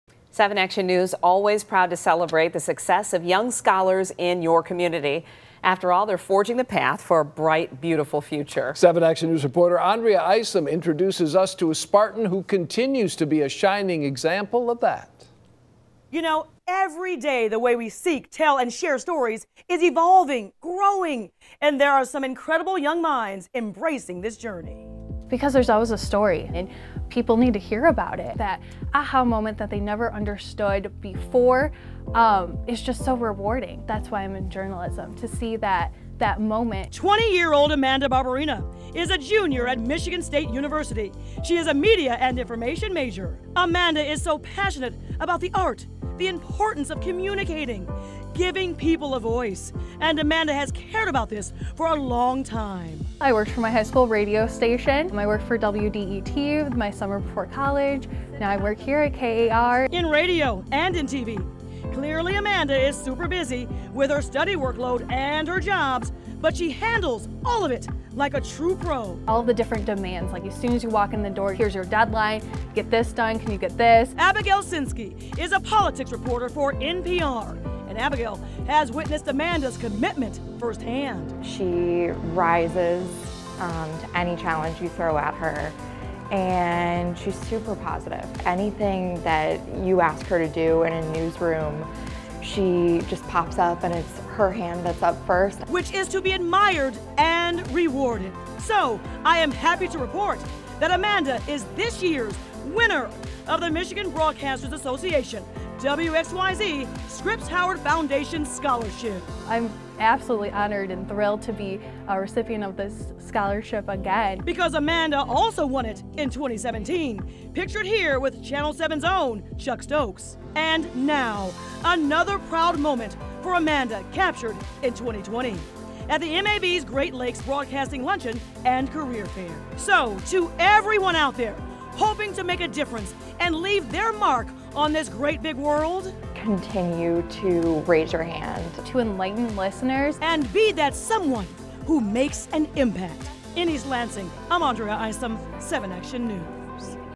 WXYZ salutes Amanda Barberena - one of journalism's next rising stars
The way we seek, share, and deliver the news and information is changing every day.  There are some talented young people, eager to take on this incredible journey of journalism. One of them is Michigan State University's own Amanda Barberena.  Amanda is the winner of the 2020 Michigan Associations of Broadcasters/WXYZ/Scripps Howard Foundation Scholarship.